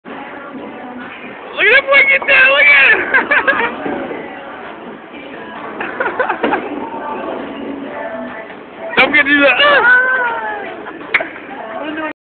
Sdsmith getting down
Bowling night... had 1 of my mj shirts on and was feeling it!